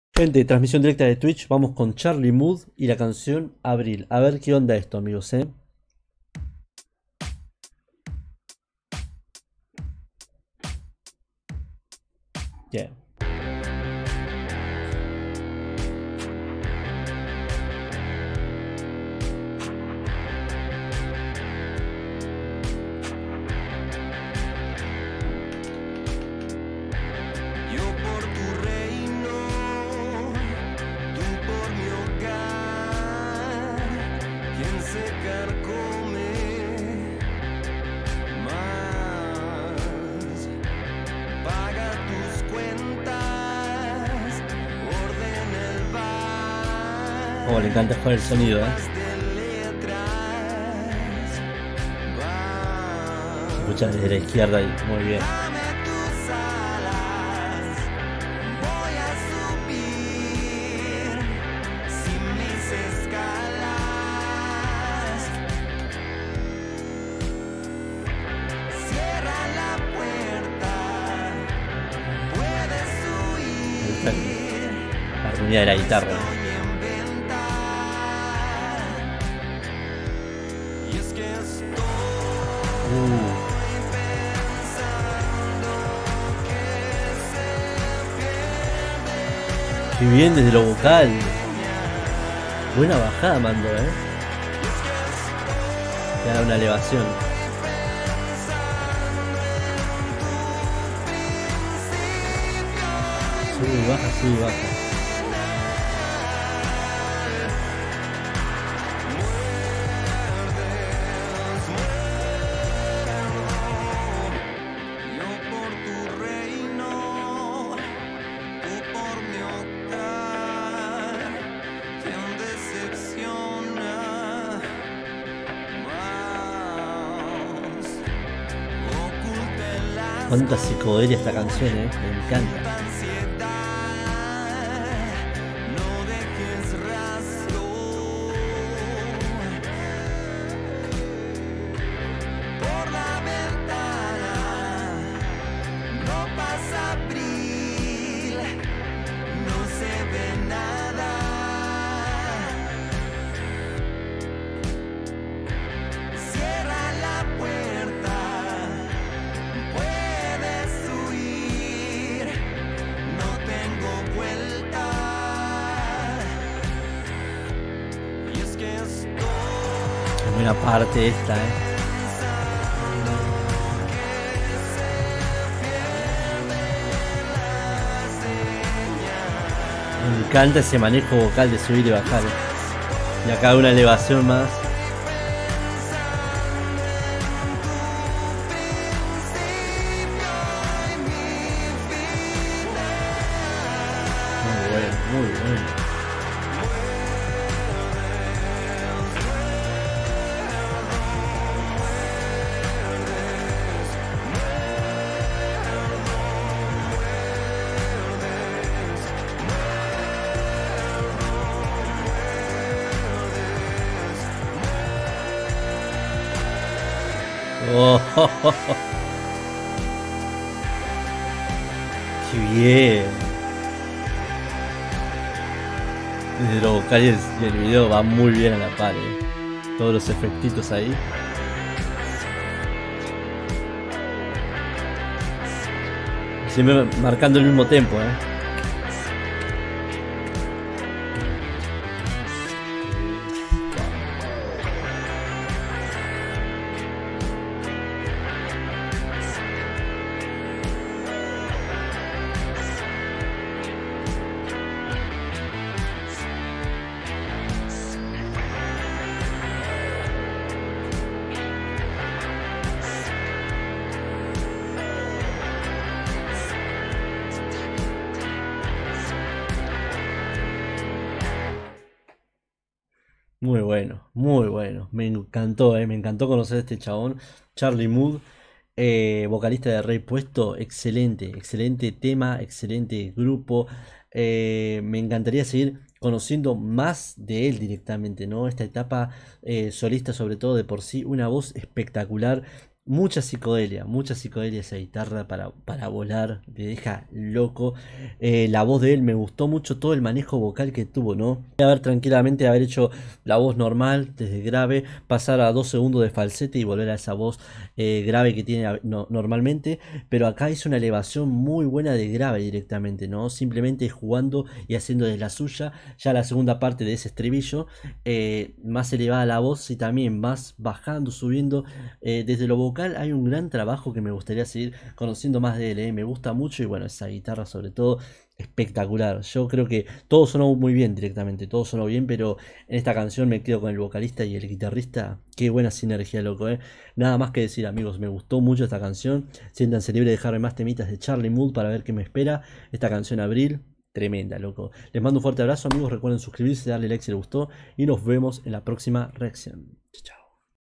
🇨🇱🇦🇷 ESCUCHO POR PRIMERA VEZ A: CHARLIE MUDD 🤯
Bien amigos, hoy volvemos con los hermanos de Chile de la mano de un artista que llega al canal.
Estoy hablando nada más y nada menos que: Charlie Mudd 🔥😎

Hasta aca mi reaccion amigos, me encanto conocerlo a él y la psicodelia de esta cancion
Sientanse libres de dejarme mas canciones de Charlie Mudd para ver que más me espera! 🤩

¡Saludos! 🤗